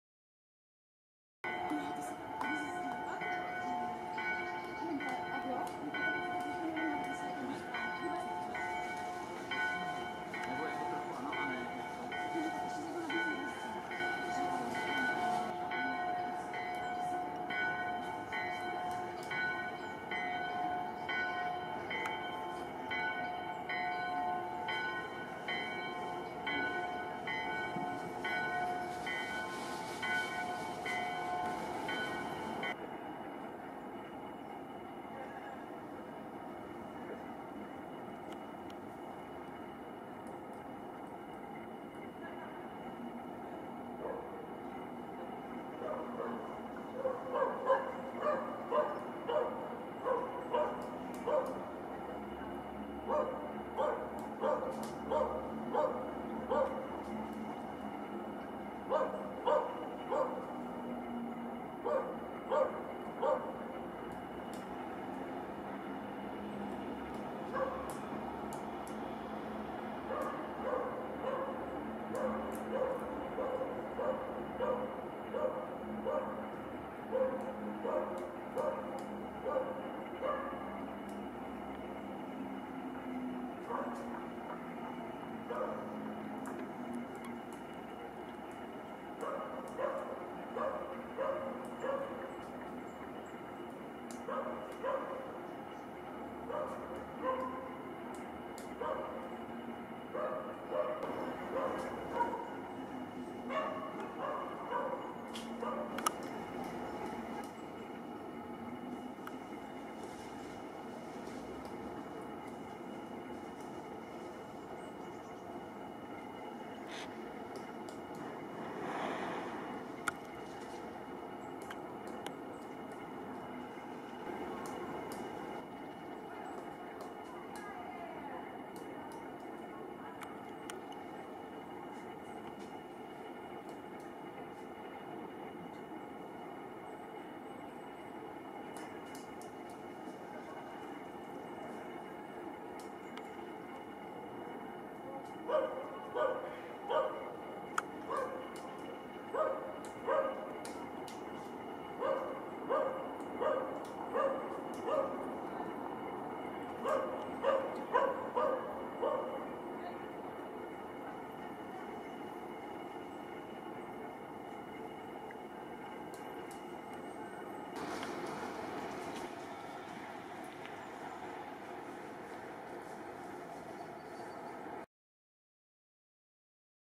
Pito negro/Peto negro/Black woodpecker (Dryocopus martius), in Praga, Czech Republic
Female of Black woodpecker (Dryocopus martius) feeding on a tree, in Czech Republic.